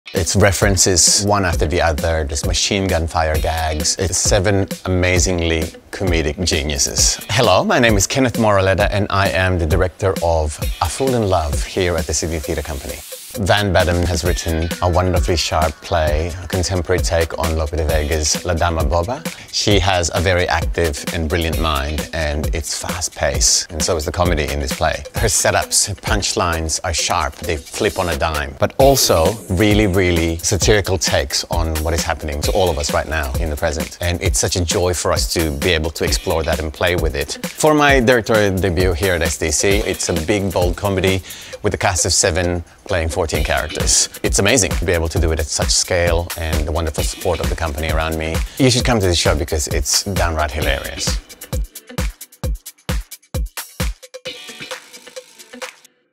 A Fool in Love | Resident Director Kenneth Moraleda
"For my directorial debut here at STC, it's a big, bold comedy with a cast of seven playing 14 characters."

The delightfully camp and vibrant production of A FOOL IN LOVE opens tomorrow at The Wharf led by Resident Director Kenneth Moraleda in his exciting STC directorial debut.